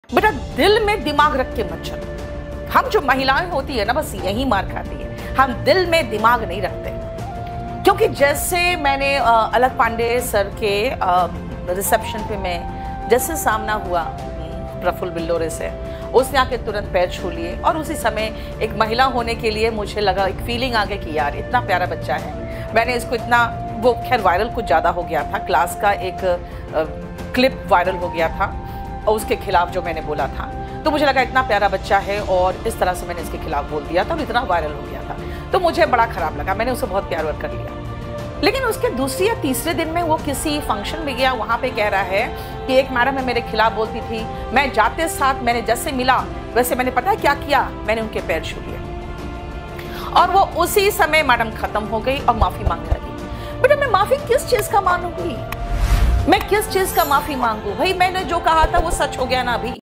MBA Chaiwala (Praful Billore) झूठ बोलते हुए रंगे हथों पकड़ा गया By Neetu Singh Mam
1st Channel :  @KD_LIVE
2nd channel : @ssckdlive
Neetu Mam Official Channel :  @NeetuSinghEnglish